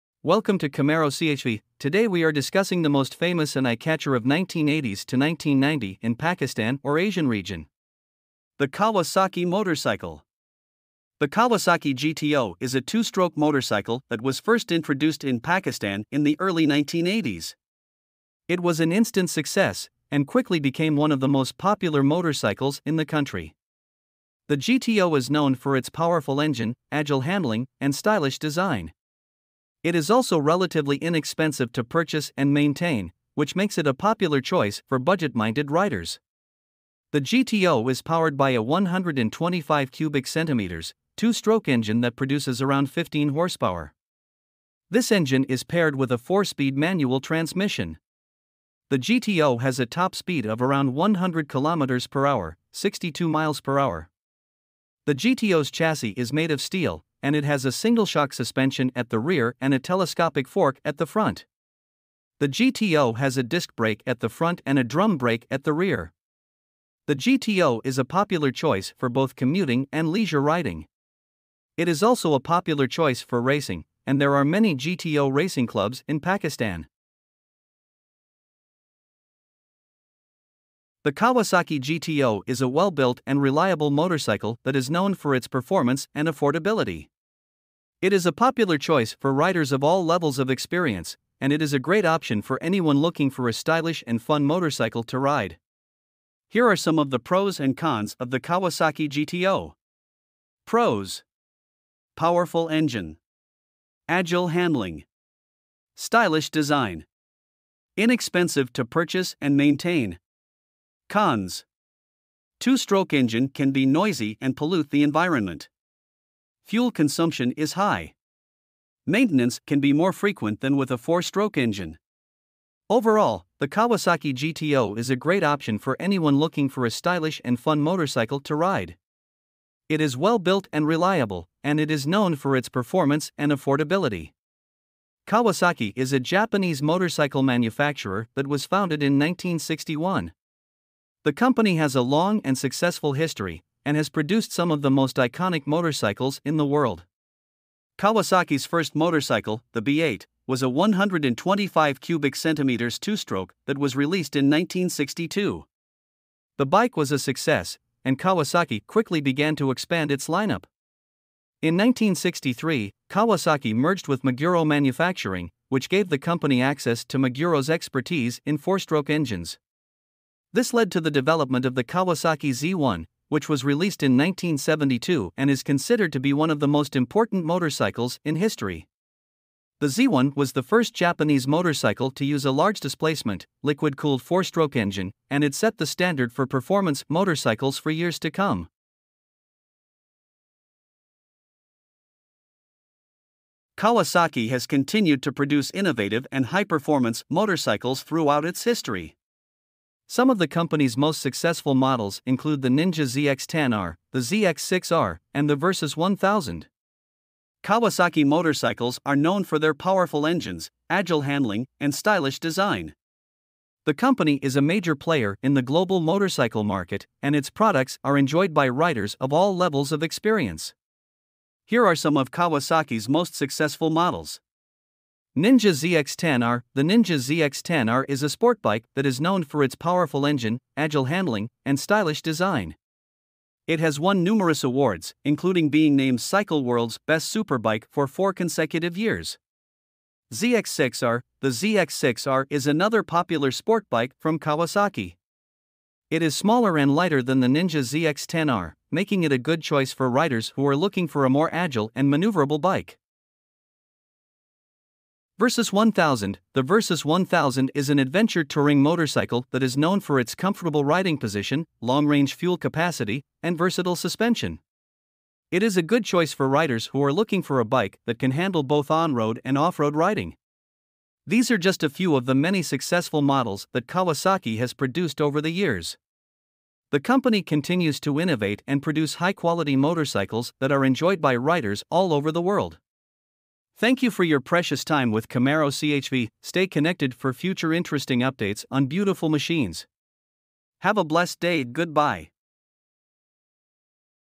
Kawasaki GTO 125 to Kawasaki Ninja | History and New Models
Kawasaki is a Japanese motorcycle manufacturer that was founded in 1961. The company has a long and successful history, and has produced some of the most iconic motorcycles in the world.
Kawasaki's first motorcycle, the B8, was a 125cc two-stroke that was released in 1962. The bike was a success, and Kawasaki quickly began to expand its lineup. In 1963, Kawasaki merged with Meguro Manufacturing, which gave the company access to Meguro's expertise in four-stroke engines. This led to the development of the Kawasaki Z1, which was released in 1972 and is considered to be one of the most important motorcycles in history. The Z1 was the first Japanese motorcycle to use a large-displacement, liquid-cooled four-stroke engine, and it set the standard for performance motorcycles for years to come.

Kawasaki Ninja story

For products